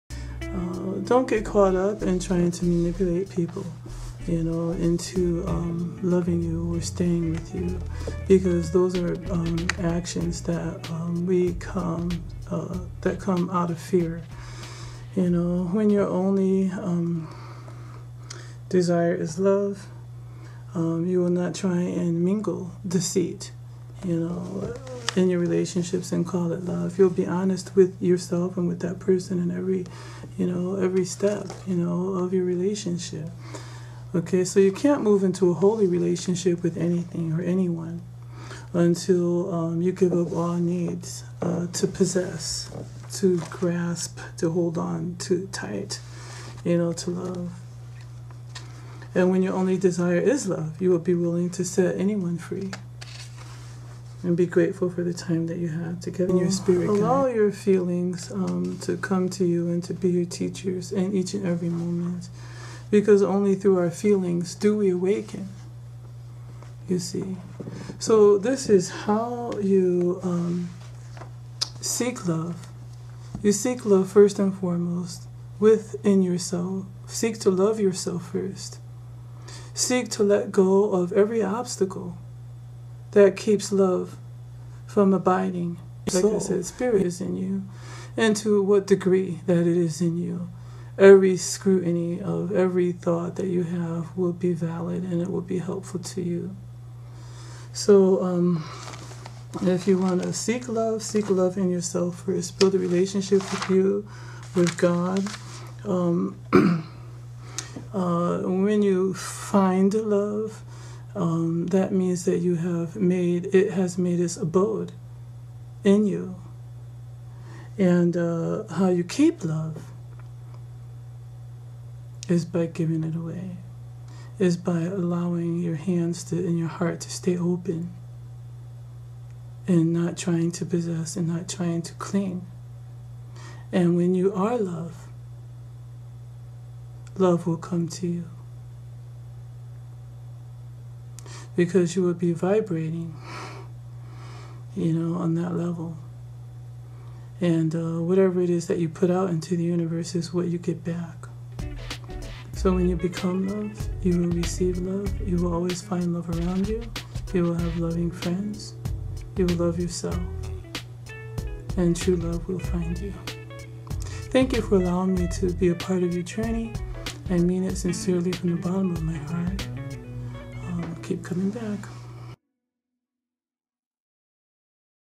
Love Lessons, Seeking, Finding and Keeping Love, 7 of 7
Learn what Scripture says about Love
The Four Seasons of Love
The Way of the Mastery on Love
Love and Fear, and
Seeking, Finding and Keeping Love

This is a series  however, each section is created to stand alone. Watch one, all, or the ones that interest you without worrying about missing info contained in the other parts.

Peace on your Journey Home~